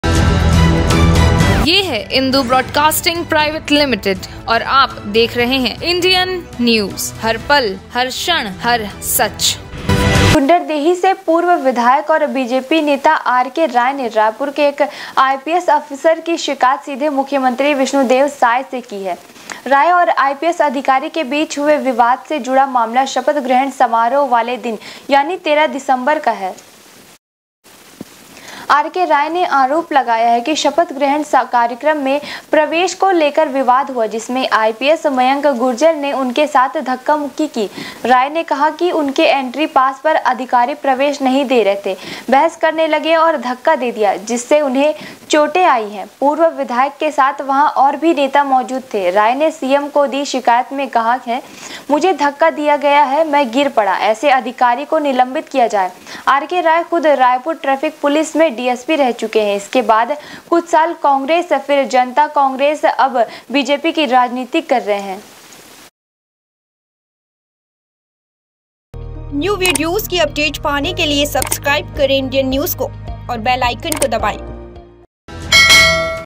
Raipur : बीजेपी नेता आर के राय ने CM विष्णु देव साय से की IPS अफसर की शिकायत l
Our Links